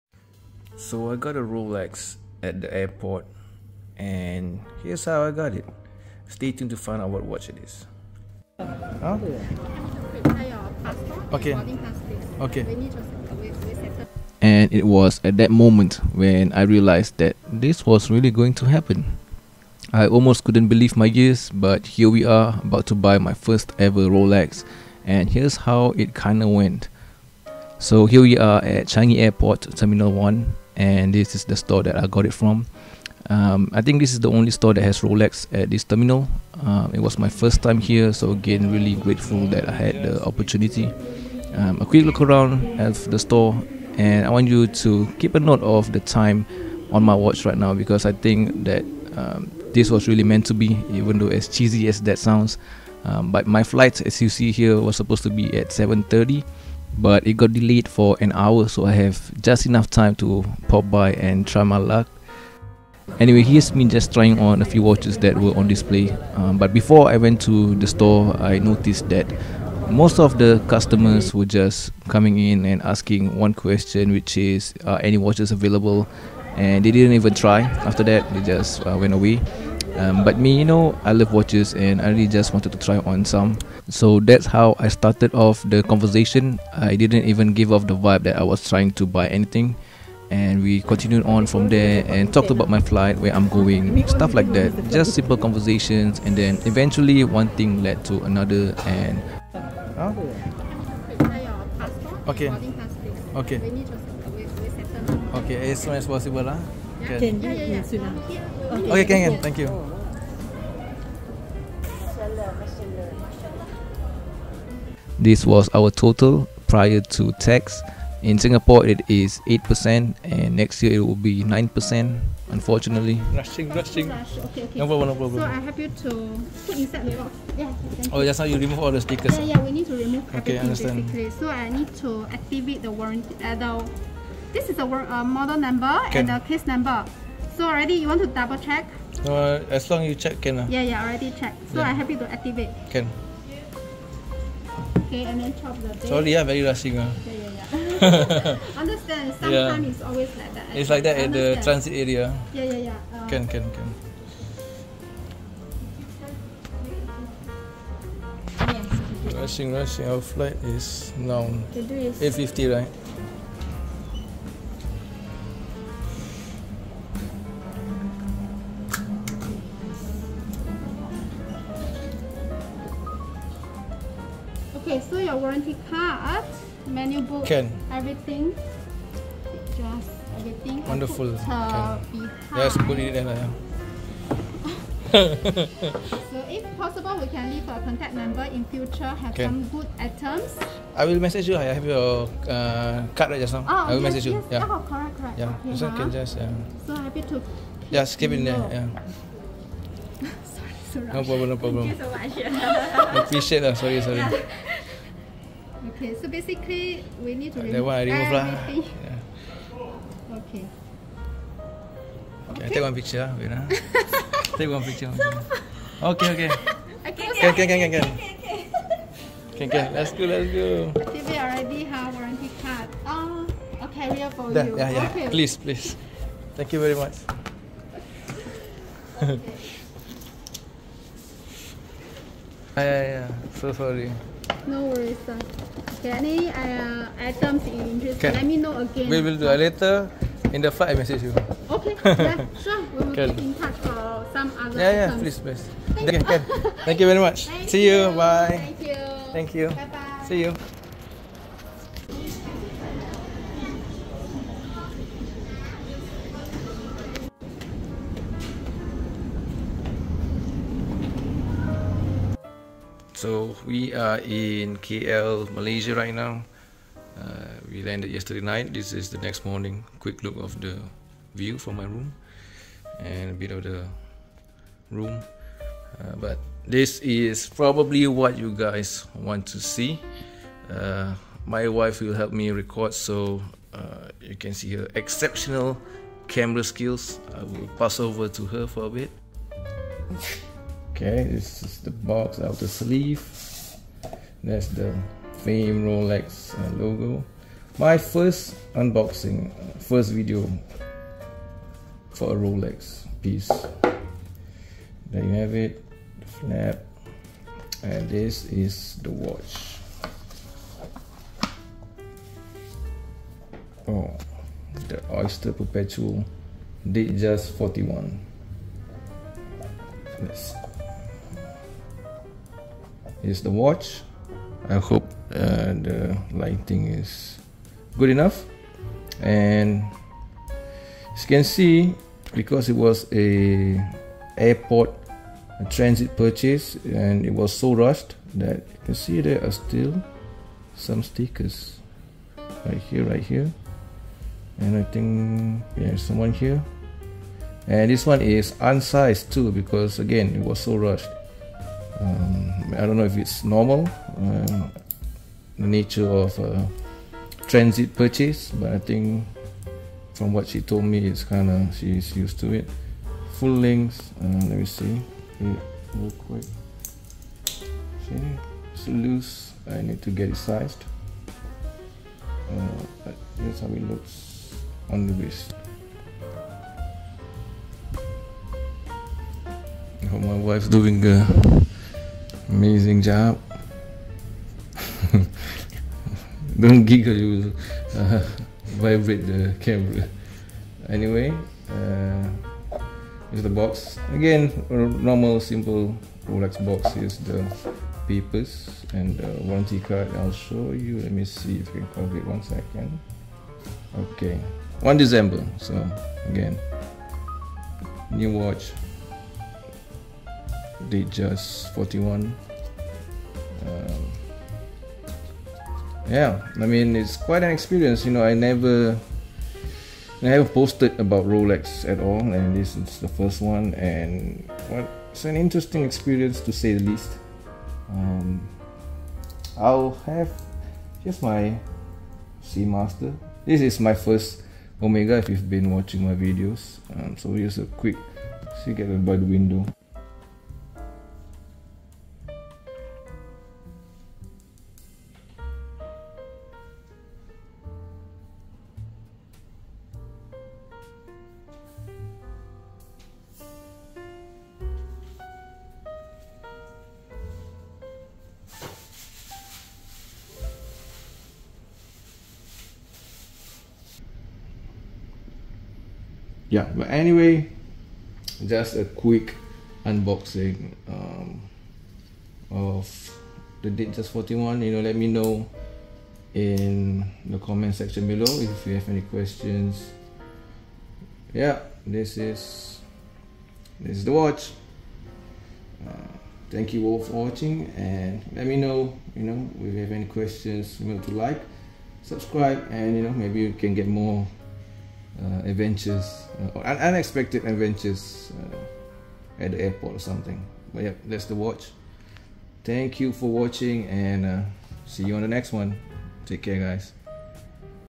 I BOUGHT A ROLEX AT THE AIRPORT DUTY FREE 2023!
Hey guys, here's how I got a Rolex at the airport!